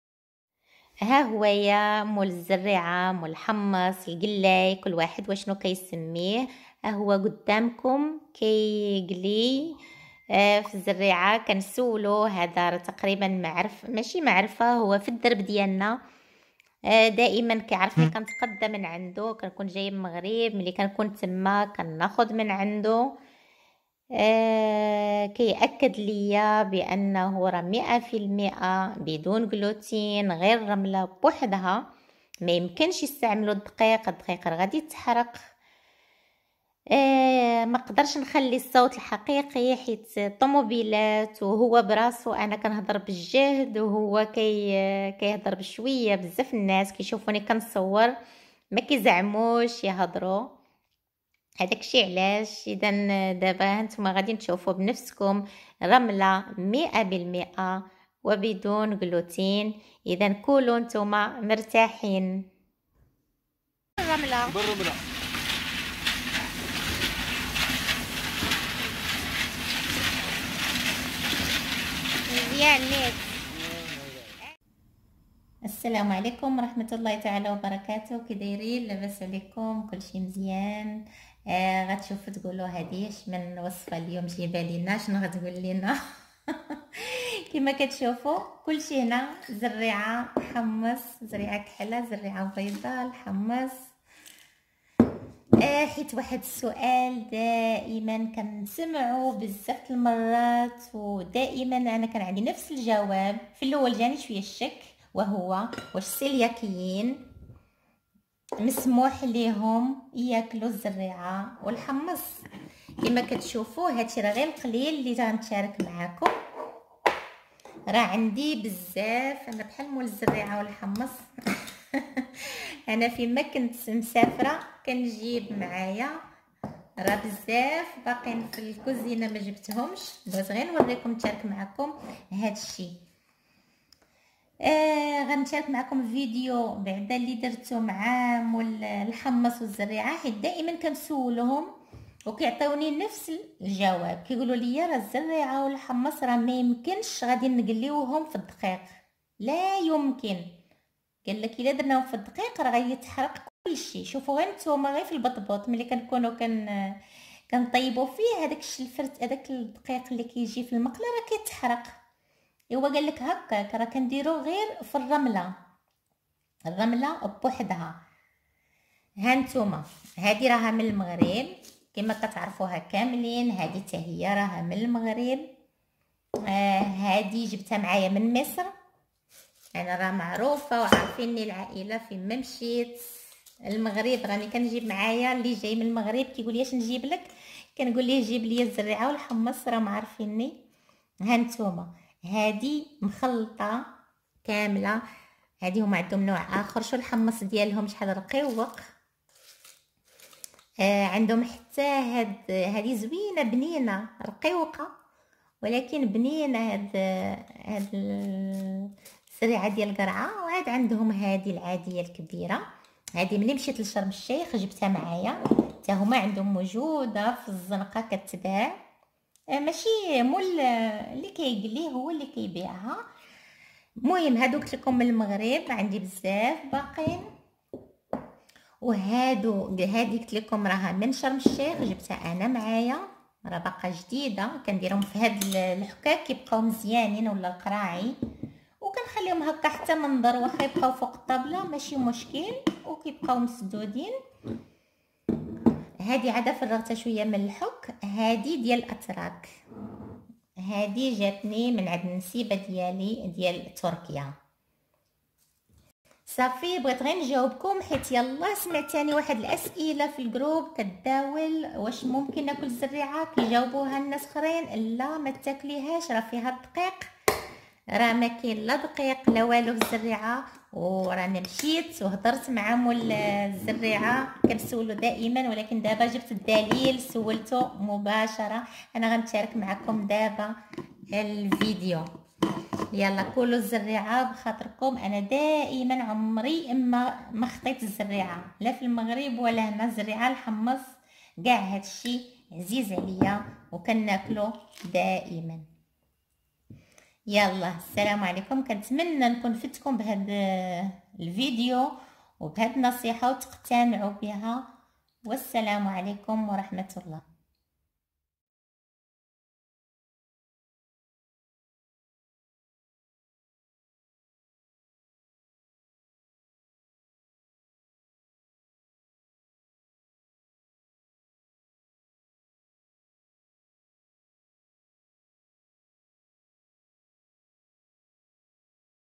الزريعة الحمص بدون غلوتين السوداء البيضاء كاوكاو من عند الgلاي بعد هاد الفديو غاد تاكلوهم وعينيكم 🙈
وداعا للشك. بدون غلوتين. بدون دقيق 😍😋